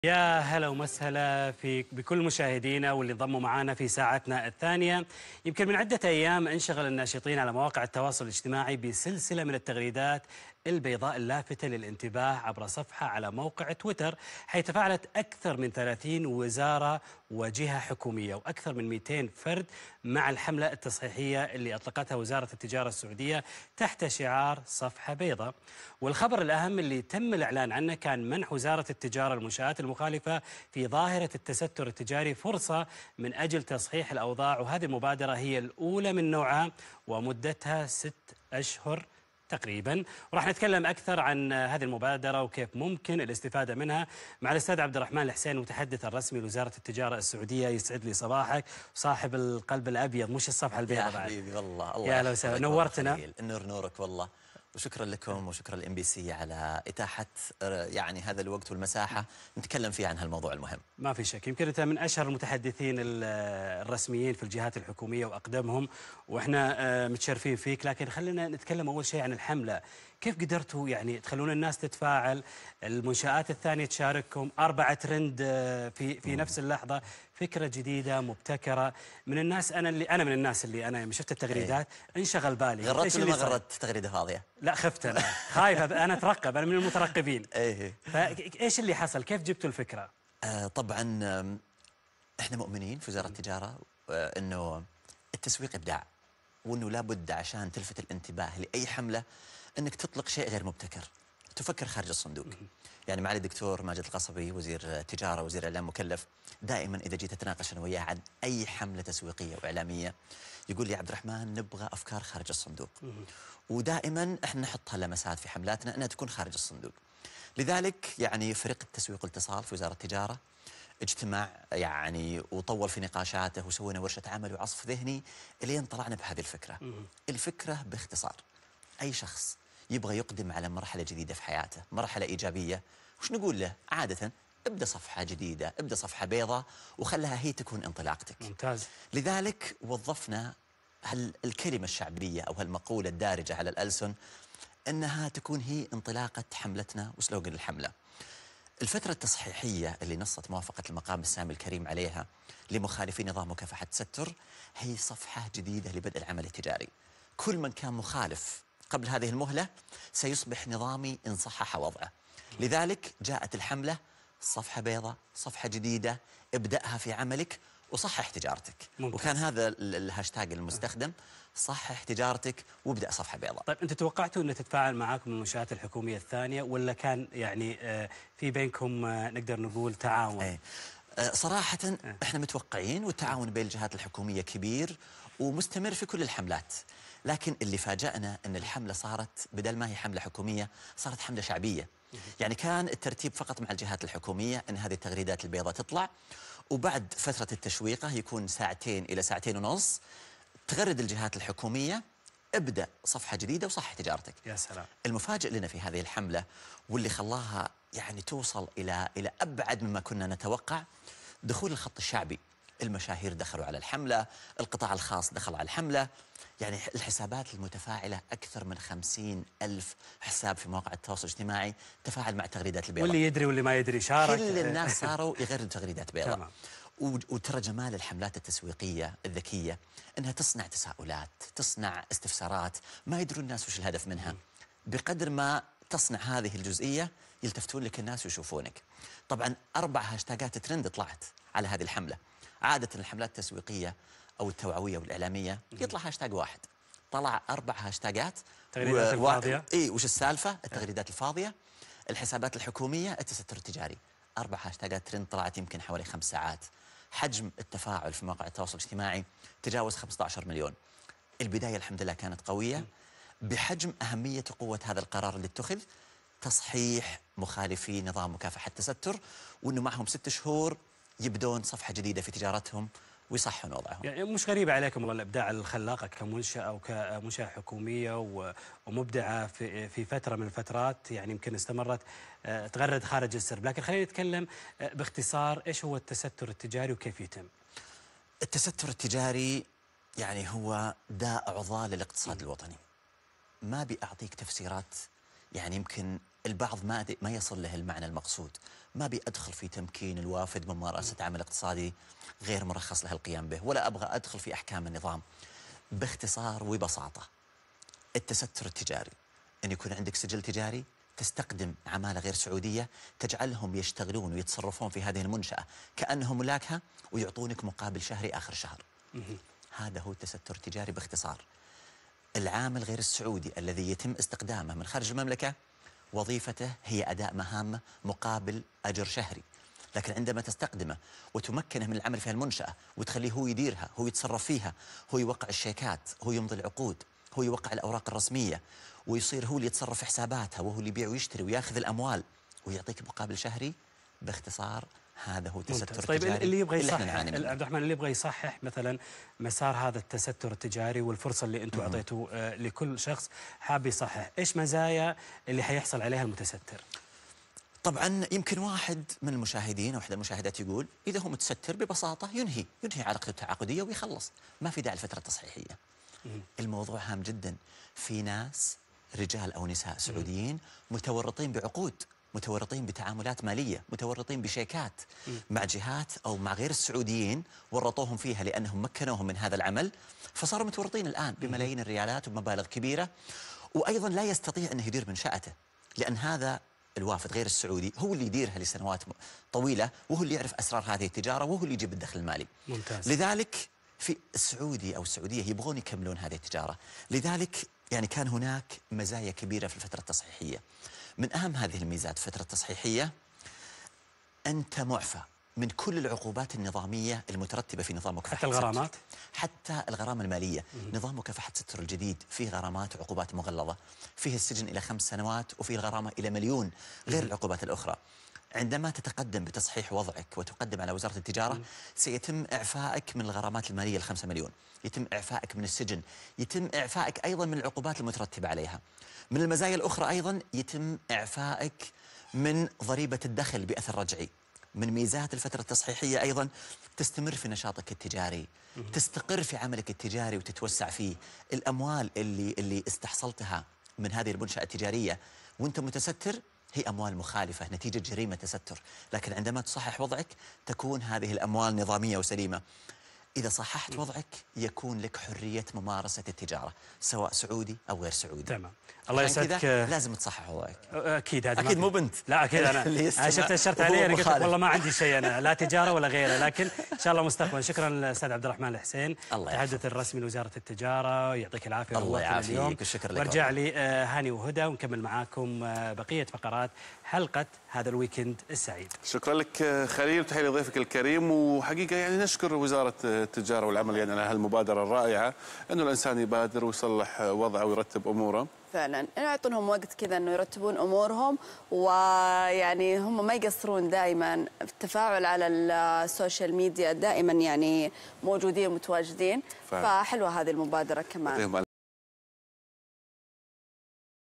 ابدأ صفحة بيضاء.. وزارة التجارة السعودية تمنح فرصة للمنشآت المخالفة لتصحيح أوضاعها
تابع جميع الحلقات على